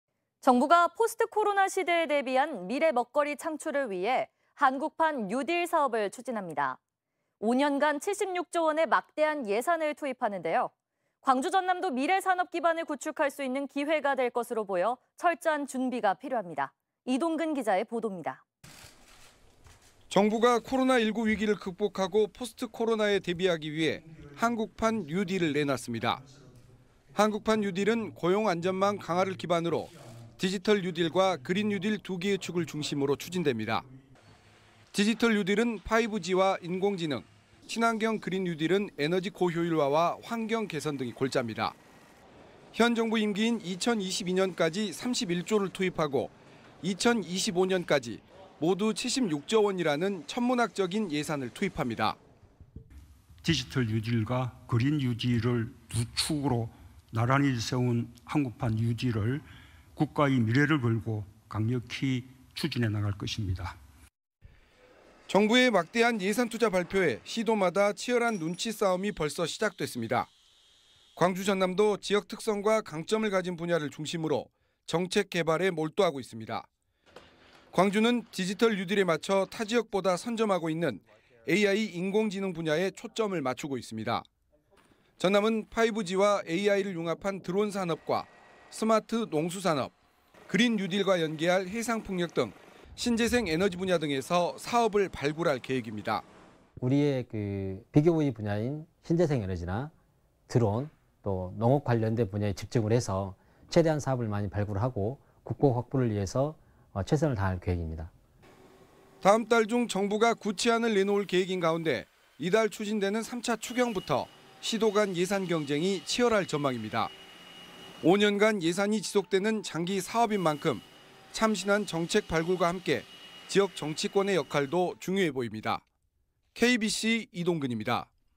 '76조 한국판 뉴딜' 광주ㆍ전남은?
【 앵커멘트 】
정부가 포스트 코로나 시대에 대비한 미래 먹거리 창출을 위해 '한국판 뉴딜' 사업을 추진합니다.

5년 간 76조 원의 막대한 예산을 투입하는데 광주ㆍ전남도 미래산업 기반을 구축할 수 있는 기회가 될 것으로 보여 철저한 준비가 필요해 보입니다.

이동근 기자의 보도입니다.